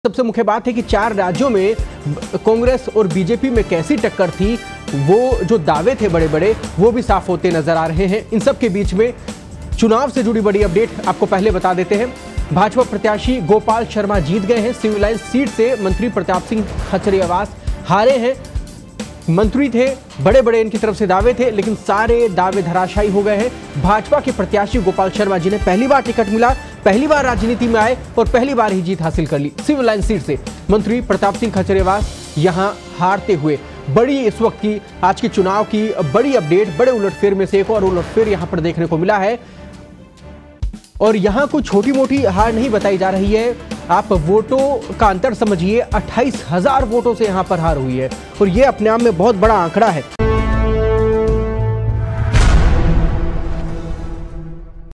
मंत्री प्रताप सिंह खाचरियावास हारे, गोपाल शर्मा जीते | Rajasthan Election Result
मंत्री प्रताप सिंह खाचरियावास हारे, गोपाल शर्मा जीते